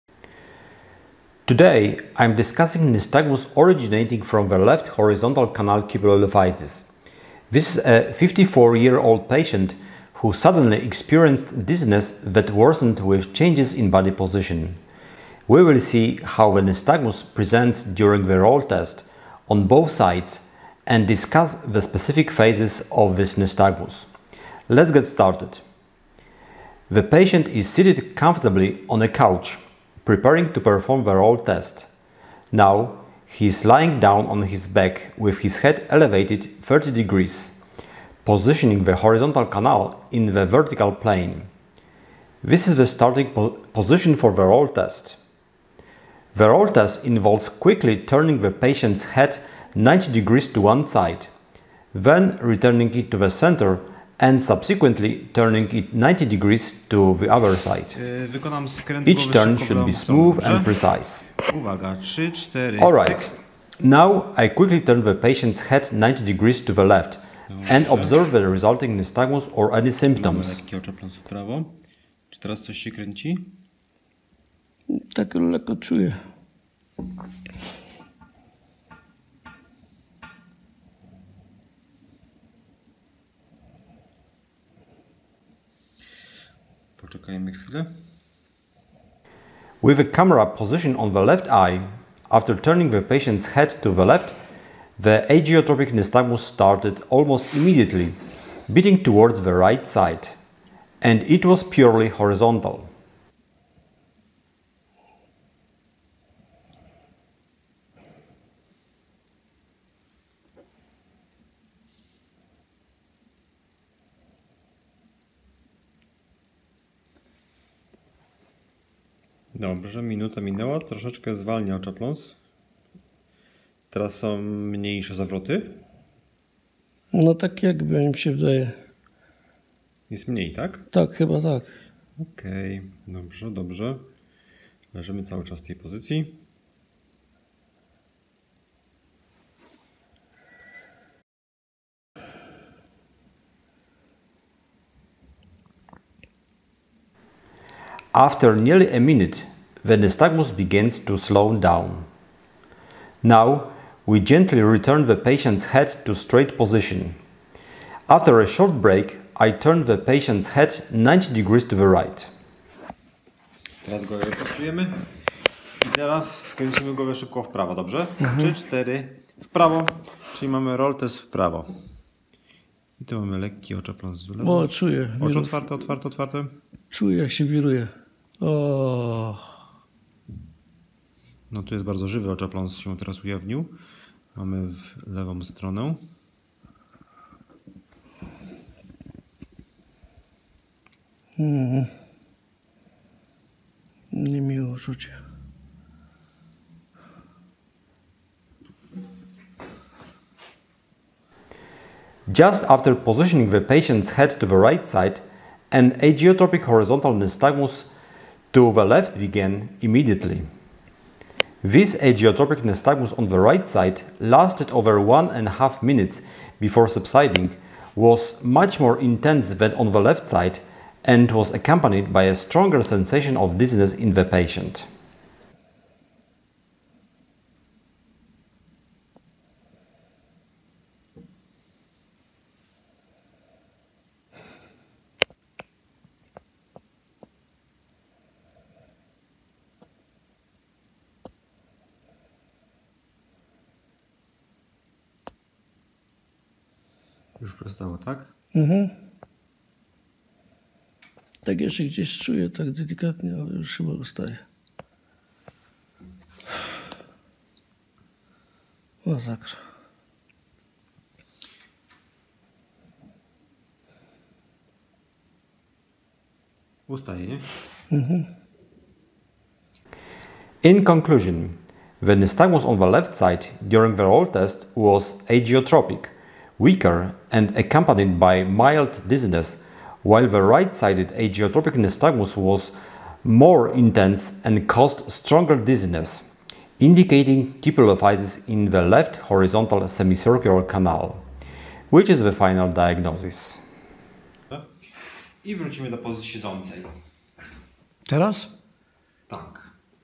Understanding nystagmus from the left horizontal canal Cupulolithisis| Practical Neuro-otology
In this video, we demonstrate nystagmus originating from the left horizontal canal Cupulolithiasis in a 54-year-old patient. The patient experienced sudden dizziness that worsened with changes in body position.

We perform the Roll Test step by step, observe how ageotropic horizontal nystagmus presents on both sides, and analyze the differences between left and right responses.

🔍 Key highlights in this video:

Roll Test procedure explained

Nystagmus observation on both sides

Difference between weaker vs stronger dizziness symptoms

Final diagnosis of left horizontal canal Cupulolithiasis

📌 Timestamps (Chapters)
00:01 – Introduction & patient case overview
00:39 – Starting position for the Roll Test
01:35 – Roll Test to the left (ageotropic nystagmus observed)
02:44 – Roll Test to the right (stronger ageotropic nystagmus)
03:40 – Comparison of left vs right side response
04:56 – Final diagnosis: Left Horizontal Canal Cupulolithiasis